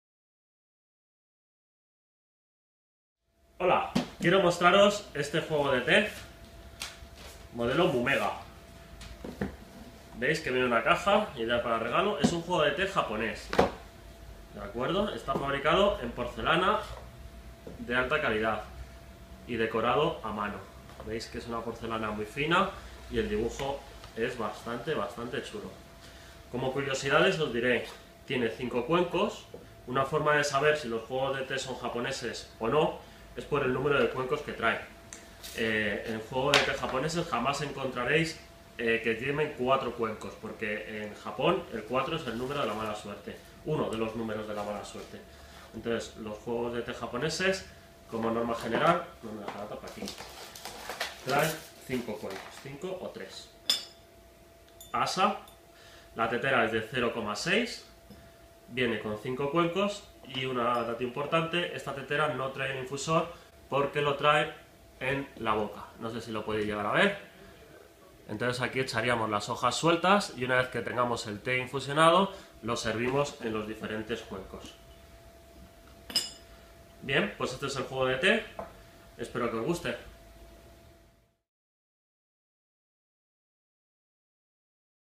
Juego de Té japonés Mumega
Te mostramos cómo es este Juego de Té fabricado en fina porcelana japonesa decorado a mano con 5 cuencos.